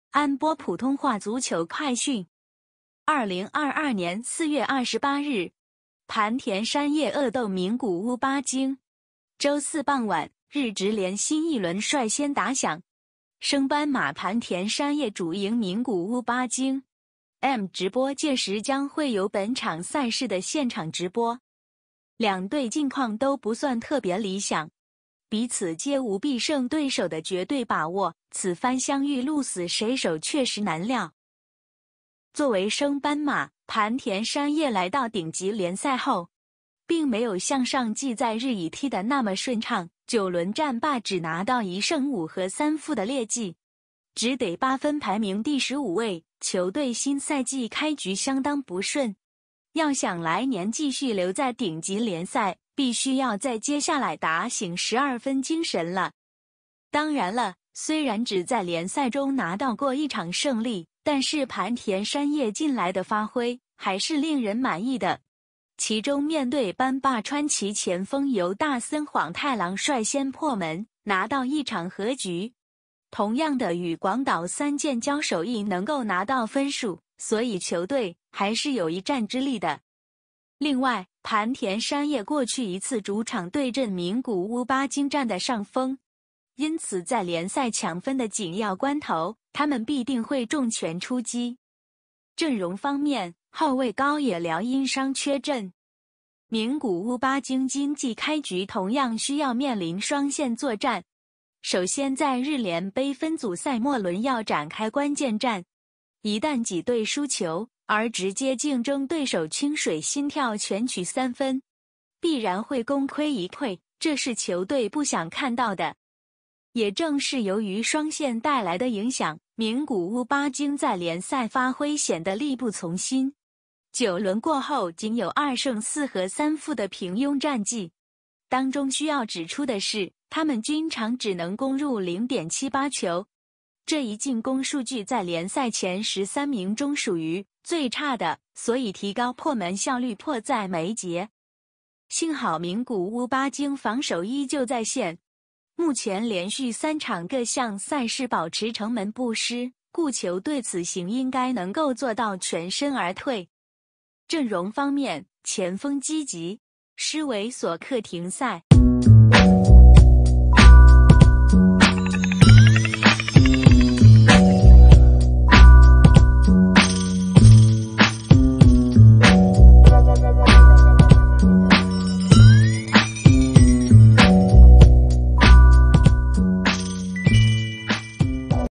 (普)【日职联-赛前新闻】2022-04-28 磐田山叶 VS 名古屋八鲸 | 磐田山叶恶斗名古屋八鲸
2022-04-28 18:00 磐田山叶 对
►立即下载M直播观看 日职联 精彩赛事

周四傍晚，日职联 新一轮率先打响，升班马#磐田山叶 主迎#名古屋八鲸 ，M直播届时将会有本场赛事的现场直播。两队近况都不算特别理想，彼此皆无必胜对手的绝对把握，此番相遇鹿死谁手确实难料。

作为升班马，磐田山叶来到顶级联赛后，并没有像上季在日乙踢得那么顺畅，9轮战罢只拿到1胜5和3负的劣绩，只得8分排名第十五位，球队新赛季开局相当不顺，要想来年继续留在顶级联赛，必须要在接下来打醒十二分精神了。当然了，虽然只在联赛中拿到过一场胜利，但是磐田山叶近来的发挥还是令人满意的，其中面对班霸川崎前锋由大森晃太郎率先破门，拿到一场和局；同样地与广岛三箭交手亦能够拿到分数，所以球队还是有一战之力的。另外，磐田山叶过去一次主场对阵名古屋八鲸占得上风，因此在联赛抢分的紧要关头，他们必定会重拳出击。阵容方面，后卫高野辽和中场山田大记皆因伤缺阵。

名古屋八鲸今季开局同样需要面临双线作战，首先在日联杯分组赛末轮要展开关键战，一旦己队输球，而直接竞争对手清水心跳全取3分，必然会功亏一篑，这是球队不想看到的。也正是由于双线带来的影响，名古屋八鲸在联赛发挥显得力不从心，9轮过后仅有2胜4和3负的平庸战绩，当中需要指出的是，他们均场只能攻入0.78球，这一进攻数据在联赛前十三名中属于最差的，所以提高破门效率迫在眉睫。幸好名古屋八鲸防守依旧在线，目前连续3场各项赛事保持城门不失，故球队此行应该能够做到全身而退。阵容方面，前锋积及.施维索克停赛。